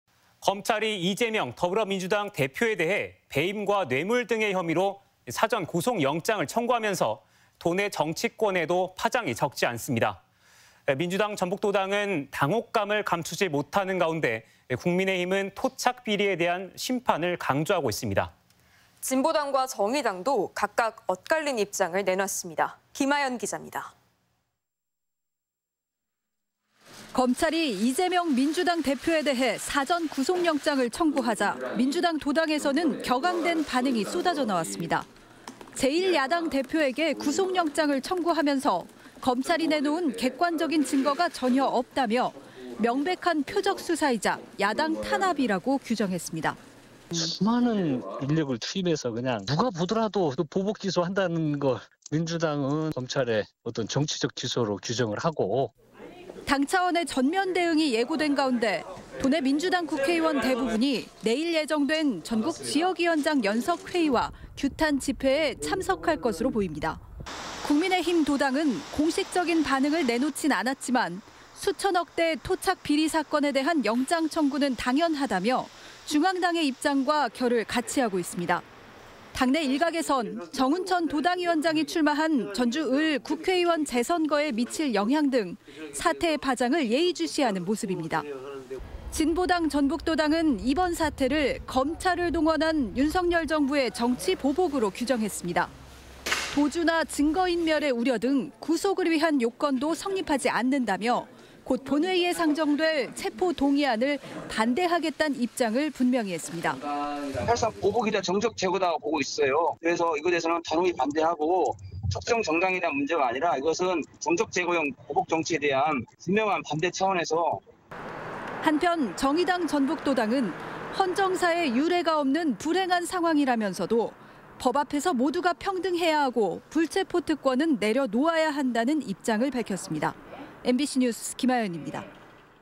"이재명 구속영장 청구".. 도내 정치권에도 파문 | 전주MBC 230216 방송
김아연 기자의 더 많은 기사를 보고싶다면?

◀앵커▶
검찰이 이재명 더불어민주당 대표에 대해 배임과 뇌물 등의 혐의로 사전 구속영장을 청구하면서 도내 정치권에도 파장이 적지 않습니다.

민주당 도당은 당혹감을 감추지 못하는 가운데 국민의힘은 토착비리에 대한 심판을 강조하고 있습니다.

진보당과 정의당도 각각 엇갈린 입장을 내놓았습니다.

김아연 기자입니다.

◀리포트▶
검찰이 이재명 민주당 대표에 대해 사전구속 영장을 청구하자 민주당 도당에서는 격앙된 반응이 쏟아져 나왔습니다.

제1야당 대표에게 구속영장을 청구하면서 검찰이 내놓은 객관적인 증거가 전혀 없다며, 명백한 표적 수사이자 야당 탄압이라고 규정했습니다.

[한병도 / 더불어민주당 전북도당위원장]
"수많은 인력을 투입해서 누가 보더라도 보복 기소 한다는 것, 민주당은 검찰의 정치적 기소로 규정을 하고..."

당 차원의 전면 대응이 예고된 가운데, 도내 의원들 대부분이 내일(17일) 예정된 전국 민주당 지역위원장 연석회의와 규탄 집회에 참석할 것으로 보입니다.

국민의힘 도당은 공식적인 반응을 내놓지 않았지만, 수천억 대 토착 비리 사건에 대한 영장 청구는 당연하다며 중앙당의 입장과 결을 같이하고 있습니다.

당내 일각에서는 정운천 도당위원장이 출마한 전주을 국회의원 재선거에 미칠 영향 등 사태의 파장을 예의주시하는 모습입니다.

진보당 전북도당은 이번 사태를 검찰을 동원한 윤석열 정부의 정치 보복으로 규정했습니다.

도주와 증거 인멸의 우려 등 구속을 위한 요건도 성립하지 않는다며, 곧 본회의에 상정될 체포 동의안을 반대하겠다는 입장을 분명히 했습니다.

[전권희 / 진보당 전북도당위원장]
"사실상 보복이자 정적 제거라고 보고 있어요. 이것에 대해서는 단호히 반대하고, 특정 정당에 대한 문제가 아니라 이것은 정적 제거용 보복 정치에 대한 분명한 반대 차원에서..."

한편 정의당 전북도당은 "헌정사에 유례가 없는 불행한 상황"이라면서도 "법 앞에서 모두가 평등해야 하고 불체포특권은 내려놓아야 한다"는 입장을 밝혔습니다.

MBC 뉴스 김아연입니다.

영상취재: 강미이